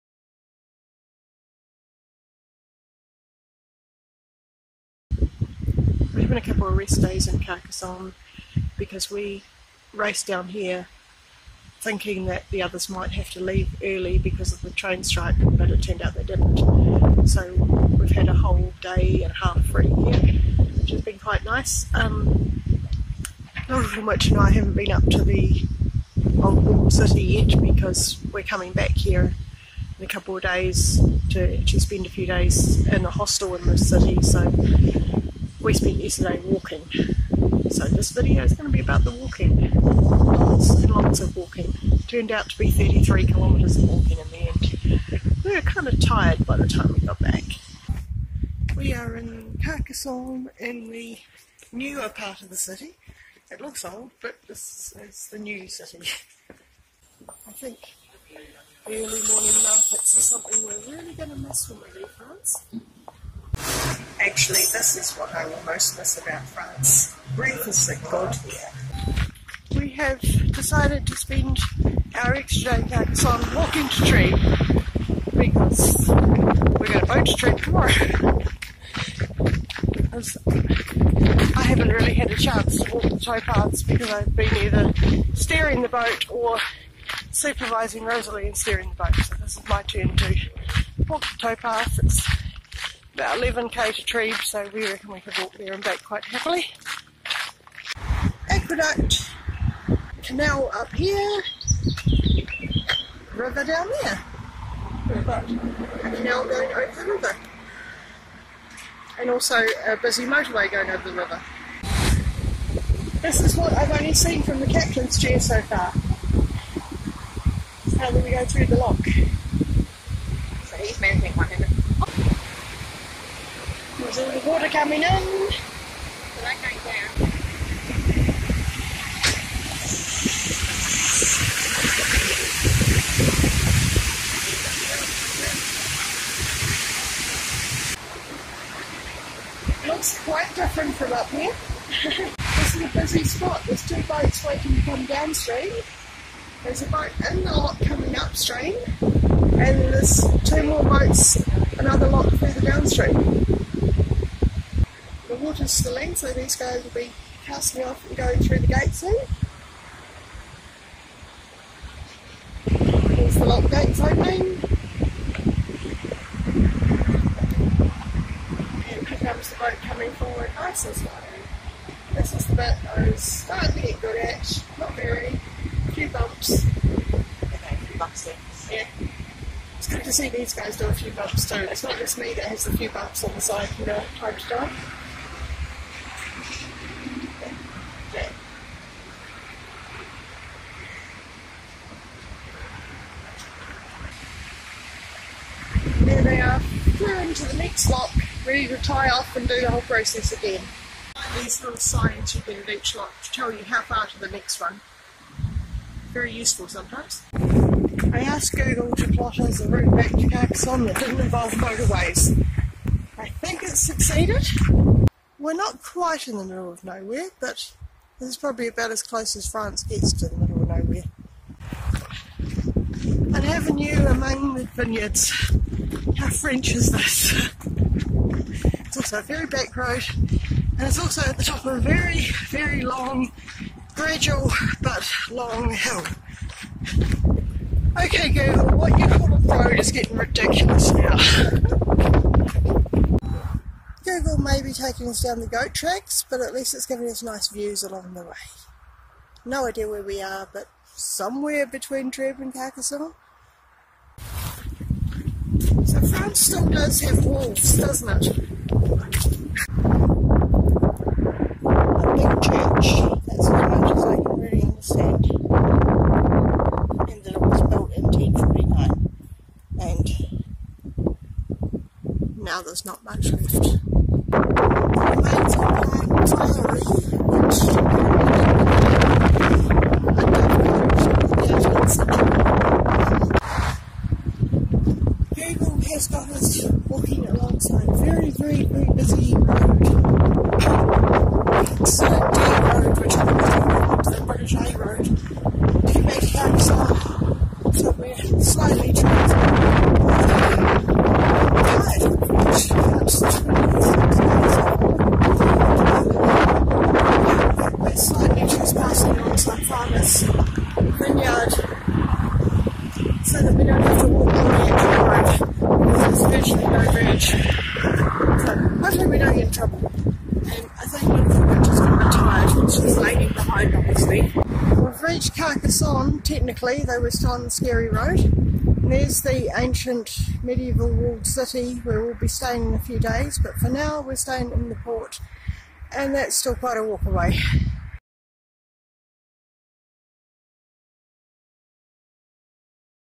Travel vlog 9: Walking to Trèbes
We took a "short" 33 km walk through the hills around Carcassonne.